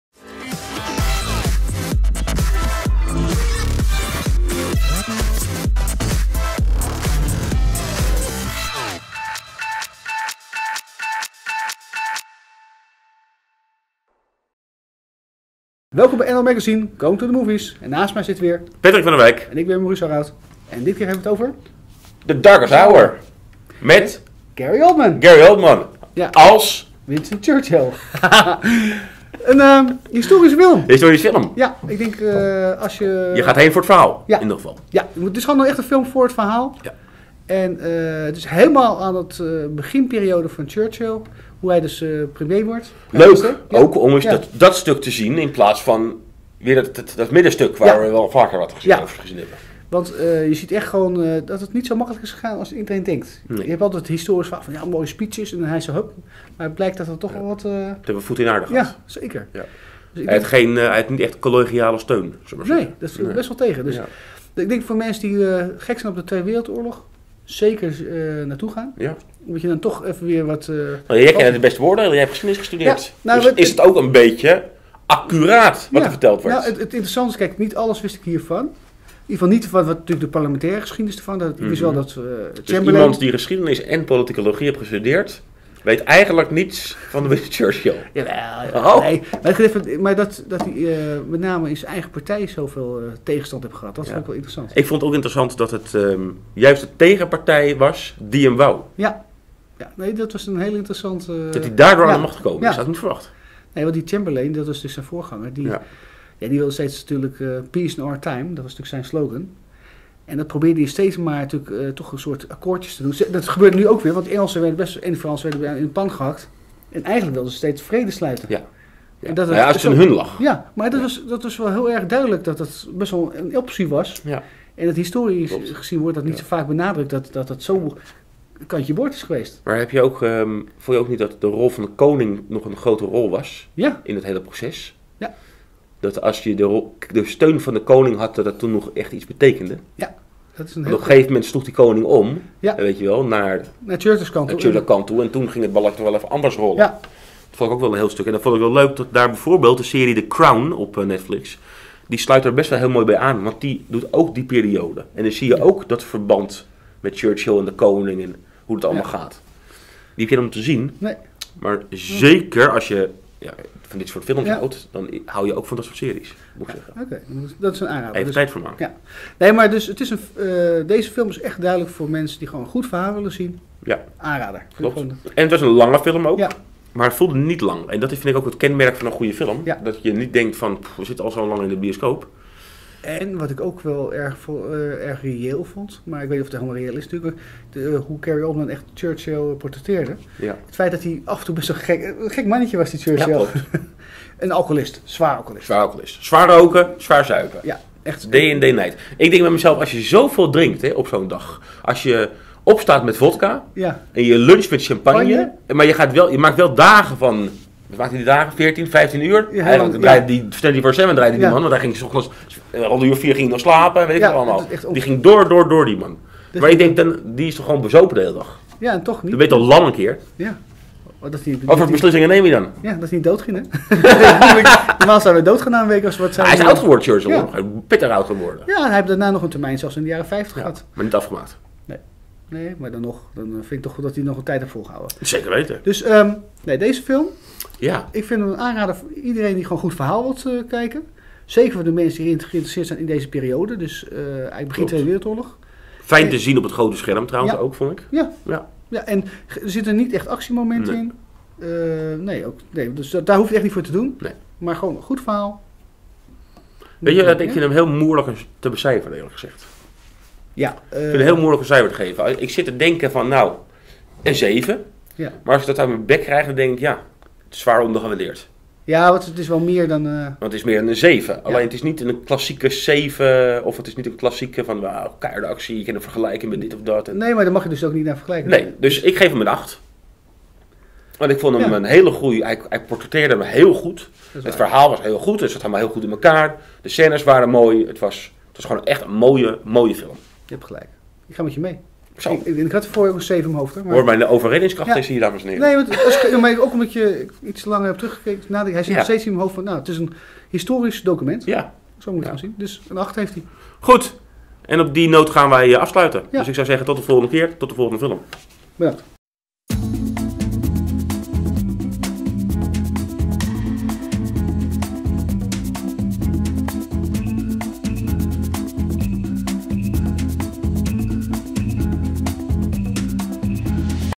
NL Movies Review Darkest Hour 2018 Nederlands
Patrick en Maurice bespreken dit historisch document: Darkest Hour met Gary Odman in de hoofdrol als Winston Churchill.

Het Verhaal: Slechts een paar dagen nadat hij premier van het Verenigd Koninkrijk is geworden, moet Winston Churchill meteen één van zijn zwaarste beproevingen doorstaan.

Hij moet kiezen of hij met Nazi Duitsland over een vredesverdrag wil onderhandelen, of dat hij voor de idealen en de vrijheid van zijn natie wil vechten.

Terwijl de niet te stoppen Nazi-troepen West-Europa binnenvallen en de dreiging van een invasie voelbaar is, heeft Churchill te maken met een onvoorbereid volk en een sceptische koning. En dan is ook nog zijn eigen politieke partij tegen hem aan het samenspannen.

Churchill moet zijn moeilijkste dieptepunt overwinnen en een natie achter zich zien te krijgen in een poging de wereldgeschiedenis te veranderen.